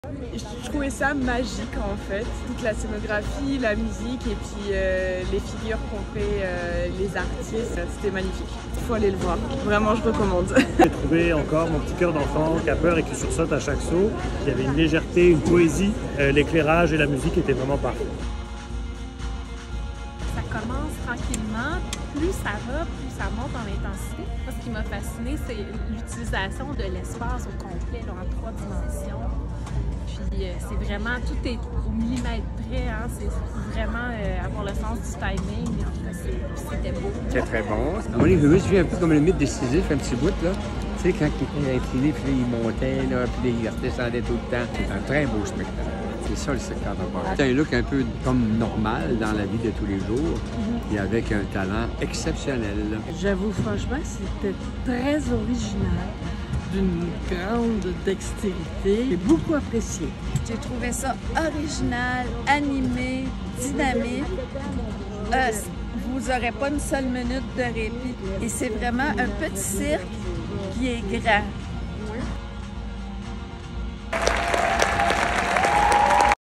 VOX POP - Esquive par Le Plus Petit Cirque du Monde
Dans ESQUIVE, six acrobates explorent et rendent hommage à une discipline : le trampoline. Témoin de la complicité qu’ils entretiennent, le public est convié à admirer leurs trajectoires allant de la légèreté grisante de l’envolée à la constante implacable qui les fait revenir au creux de la toile.

En questionnant la gravité, le poids, la suspension et les changements d’état de corps, la partition acrobatique devient un langage qui traduit les fondements intimes du cirque : l’art de l’envol et de la prise de risque, un éloge à la fragilité et à l’utopie.

ESQUIVE est une invitation à contempler le désir humain de s’élever, à ressentir son effort continu pour s’extraire de la chute.